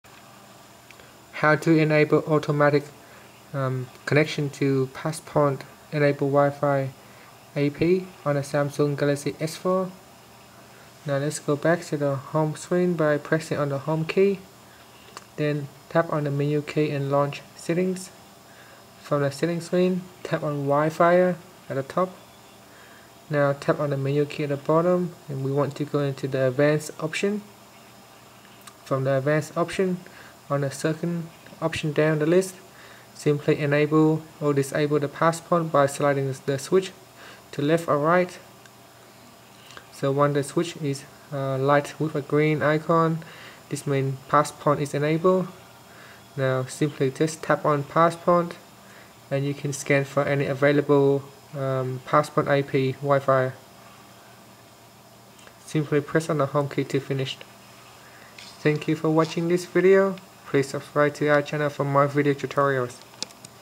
Samsung Galaxy S4: How to Enable Passpoint for Auto Wi-Fi Connection
Learn how to enable or disable auto passpoint Wi-Fi Connection